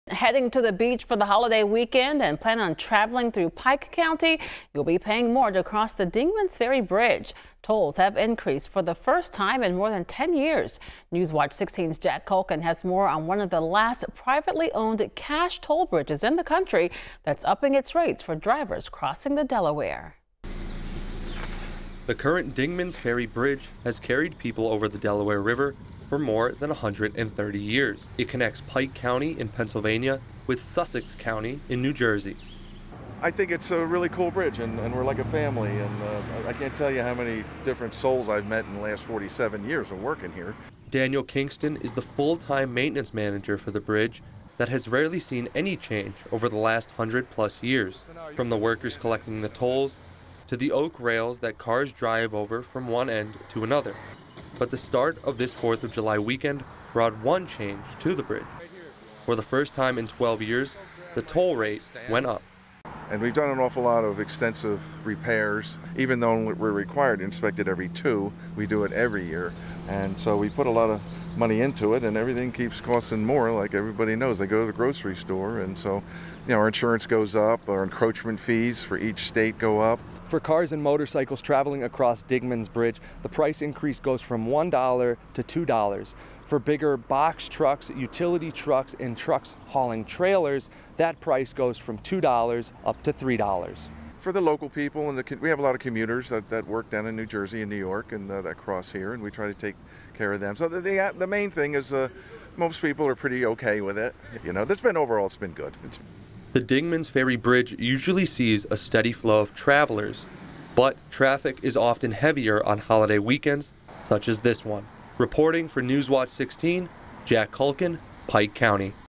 Tolls increase on privately owned bridge in Pike County
The Dingmans Ferry Bridge usually sees about 150 vehicles a day but traffic is often heavier on holiday weekends.